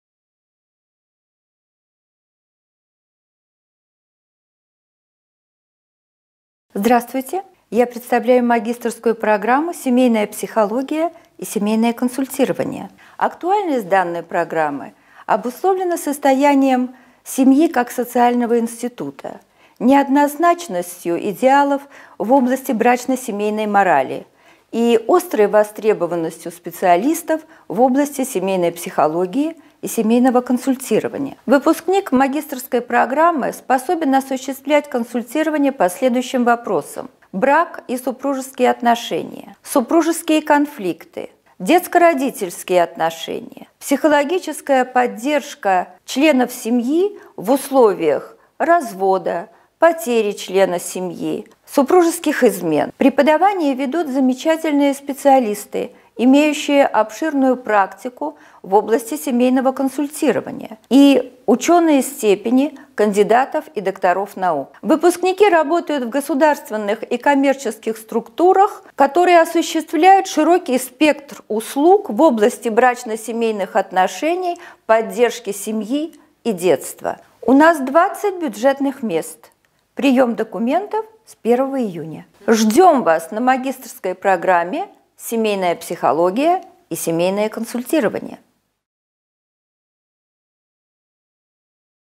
Семейная психология и семейное консультирование ⚪ Магистратура МГПУ
Магистерская программа МГПУ «Семейная психология и семейное консультирование»:

⚪ У нас преподают кандидаты и доктора наук, лучшие практики в области семейного консультирования;
⚪ Выпускники востребованы в государственных и коммерческих структурах;
⚪ 20 бюджетных мест.

Набатникова Людмила — кандидат психологических наук, доцент, автор 57 публикаций.